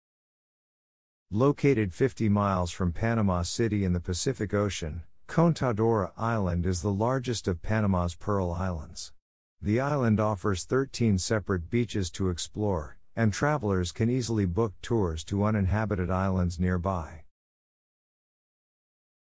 Contadora Island the Pearl Islands
Located 50 miles from Panama City in the Pacific Ocean, Contadora Island is the largest of Panama's Pearl Islands. The island offers thirteen separate beaches to explore, and travelers can easily book tours to uninhabited islands nearby..